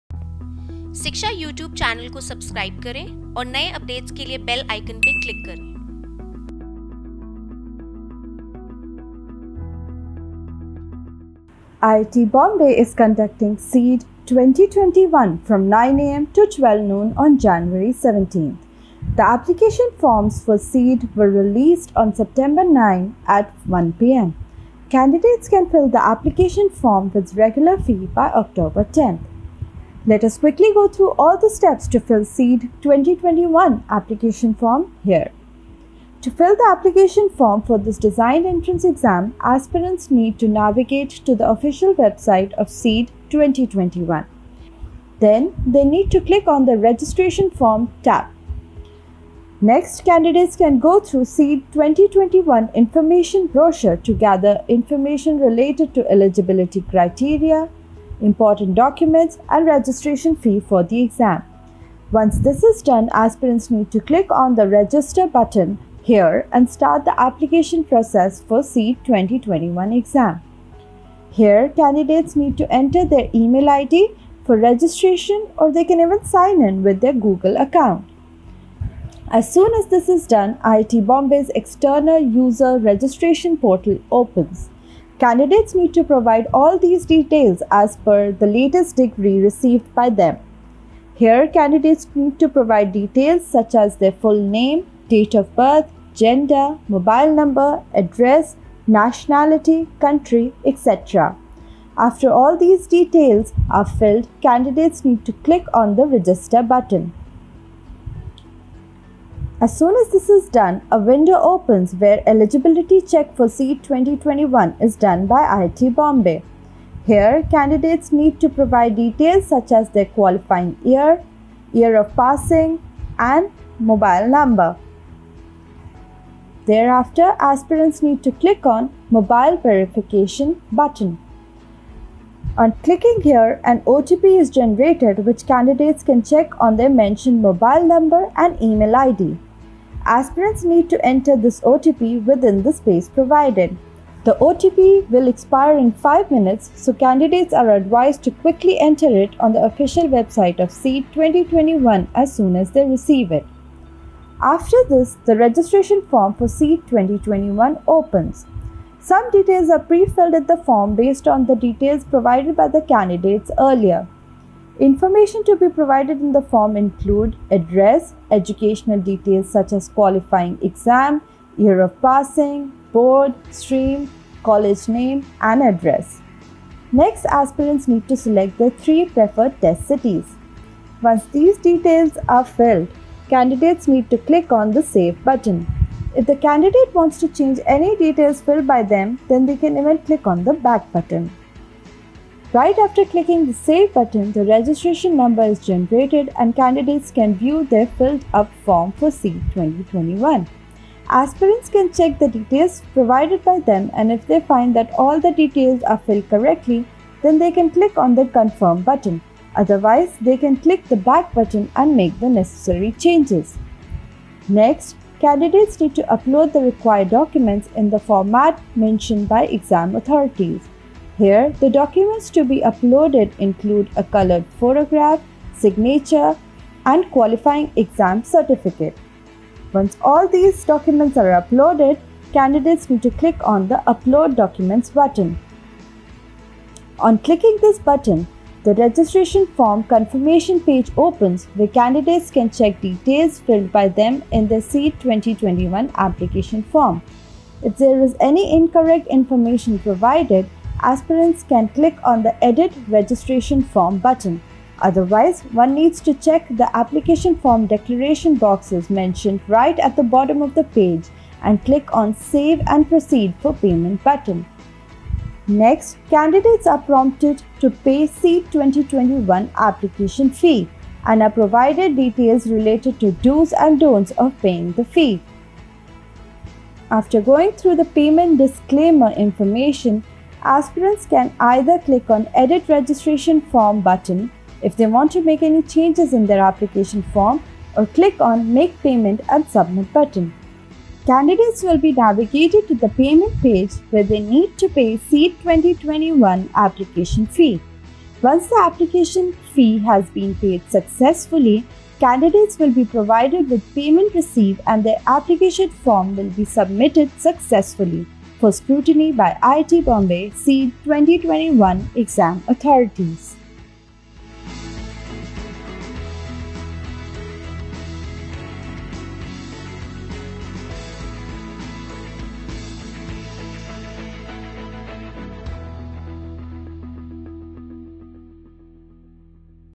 How to Fill CEED 2021 Application Form?
IIT Bombay has released CEED 2021 application form. Go through guidelines to fill CEED application form 2021. Know More